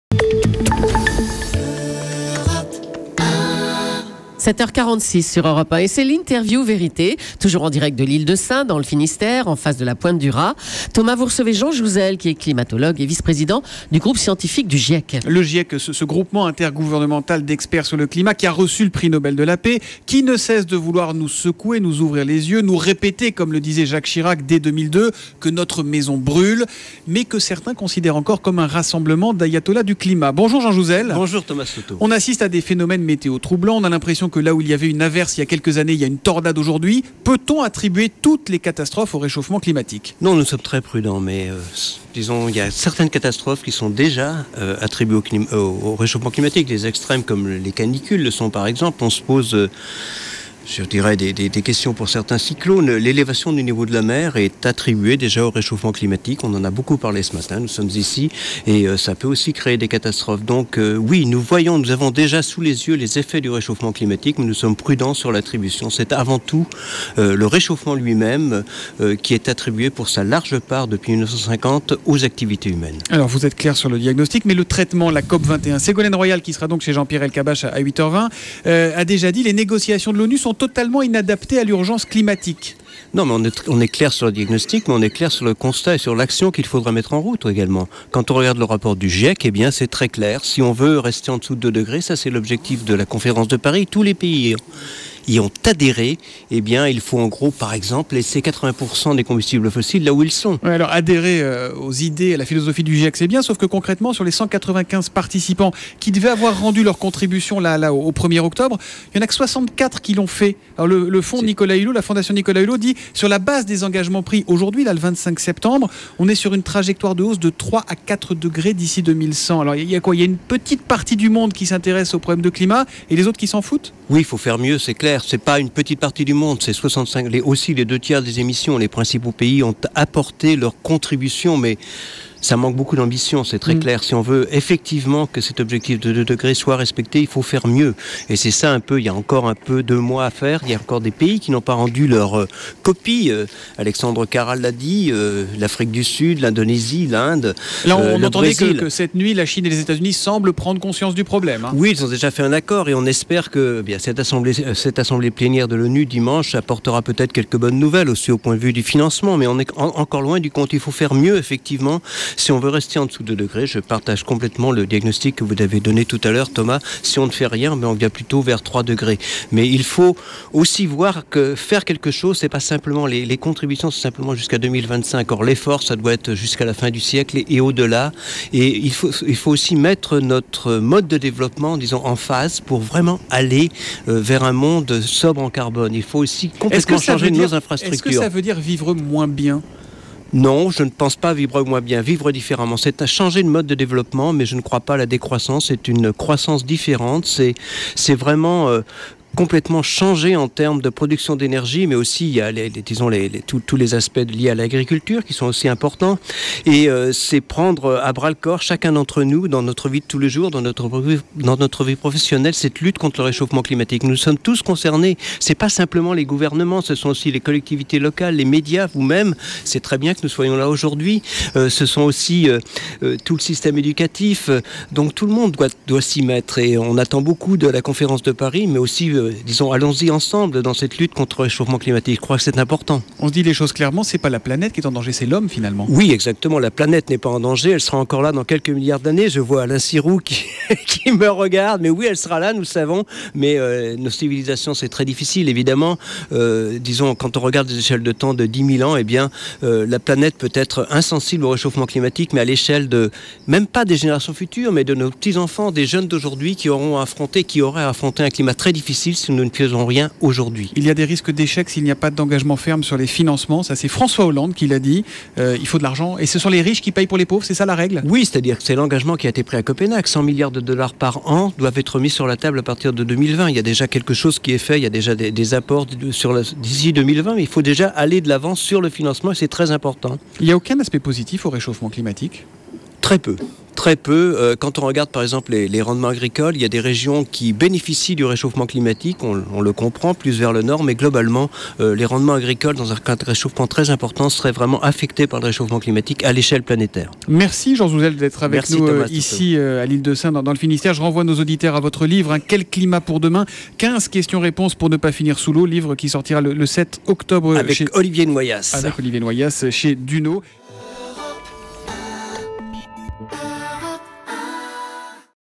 Jouzel : "Très peu d'aspects positifs au réchauffement climatique"
Jean Jouzel, climatologue et vice-président du groupe scientifique du GIEC, se confie au micro de Thomas Sotto ce vendredi.